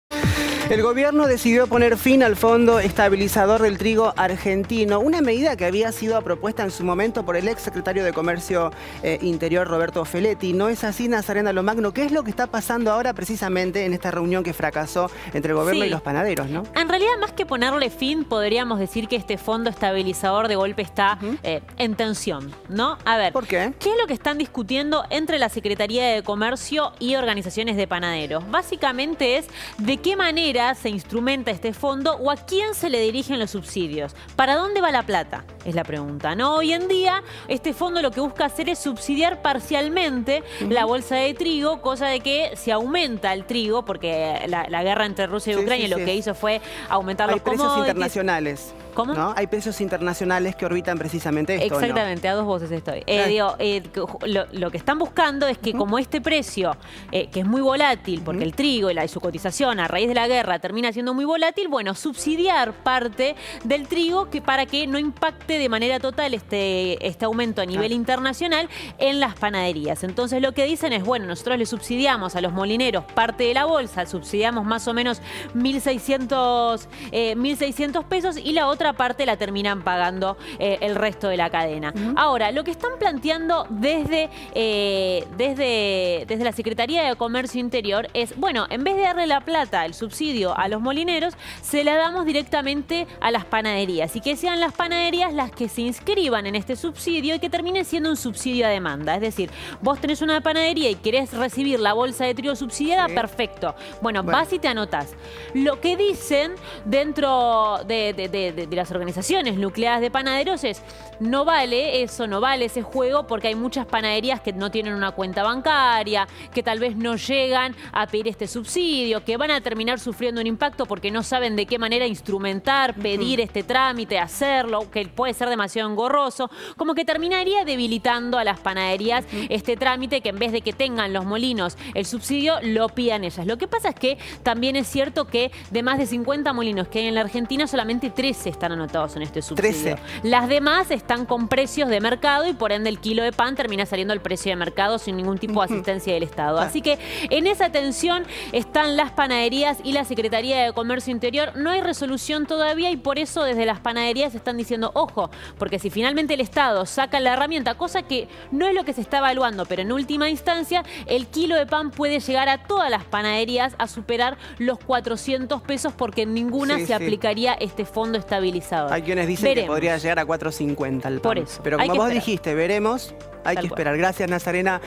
Fracasó la reunión entre panaderos y Comercio
El Gobierno decidió no continuar con el fondo estabilizador del trigo. Ahora se discute la posibilidad de subsidiar directamente a las panaderías y no a los productores. No hubo acuerdo entre Nación y panaderos.